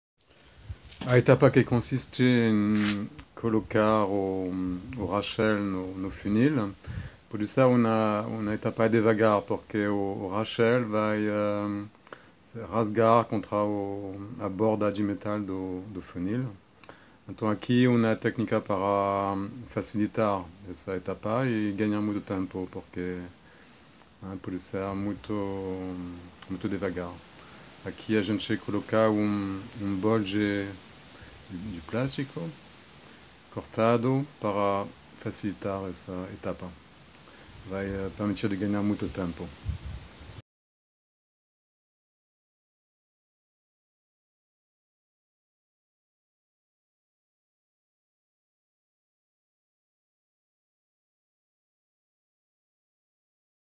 Colocar Raschel no funil
A etapa de preparação do funil para a linguiça de hiperadobe precisa bastante tempo e é delicado. O Raschel pode se rasgar facilmente com as bordas de metal do funil. Utilizando essa técnica permite de segurar o Raschel e acelerar a etapa.